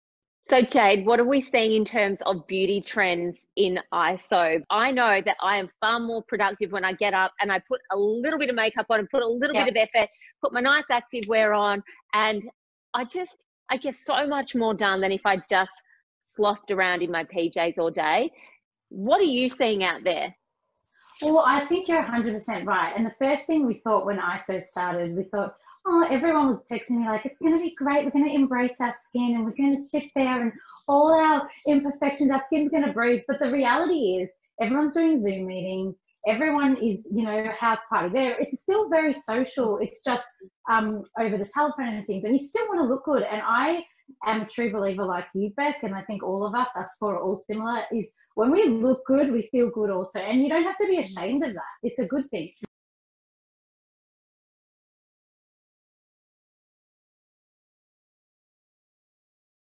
How WAG Rebecca Judd stays motivated in lockdown
The surprising trick AFL WAG Rebecca Judd uses to stay motivated with her work in lockdown... and you'll never guess what it is.
Rebecca Judd has made balancing home-schooling her four children and managing her thriving activewear label, Jaggad, look easy.